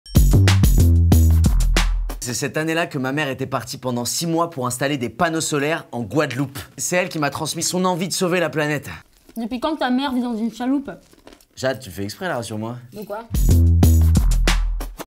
T'ES REFAIT : La capsule - Episode 25
Noah a toujours voulu avoir un métier qui a du sens. Comme sa mère, il fait un métier qui a un vrai impact sur l’environnement. Et cette voie, il y pense depuis son plus jeune âge, la preuve en vidéo !

➡️ Alors si toi aussi tu as envie de t’engager dans un parcours de formation liées à la rénovation énergétique, viens te pré-inscrire sur le

T'ES REFAIT! est une opération de recrutement digitale aux formations de la rénovation énergétique des bâtiments, initiée conjointement par l'@ademe et le @3cabtp